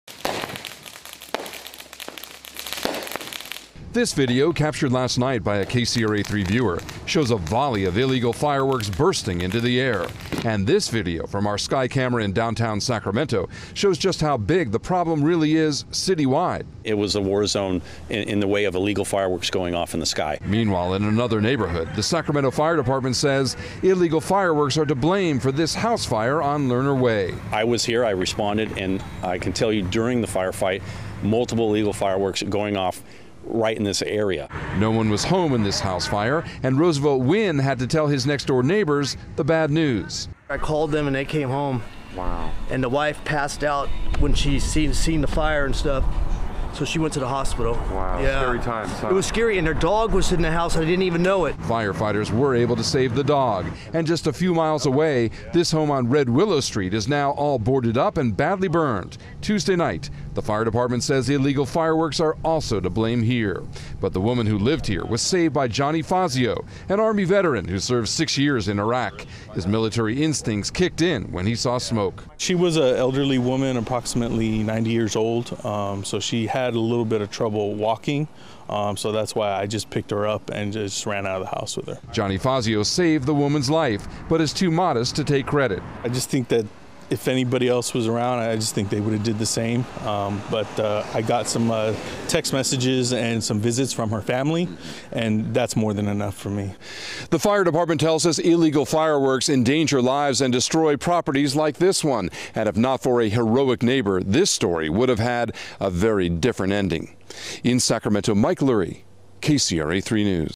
16 arrested, 2,300 pounds of illegal fireworks seized in Sacramento
Sixteen people were arrested and more than 2,300 pounds of illegal fireworks were seized -- 10 times more than last year -- by a Sacramento police fireworks task force and ATF agents over the Fourth of July. In addition, at least two homes were damaged because of the use of illegal fireworks that lit up the night sky.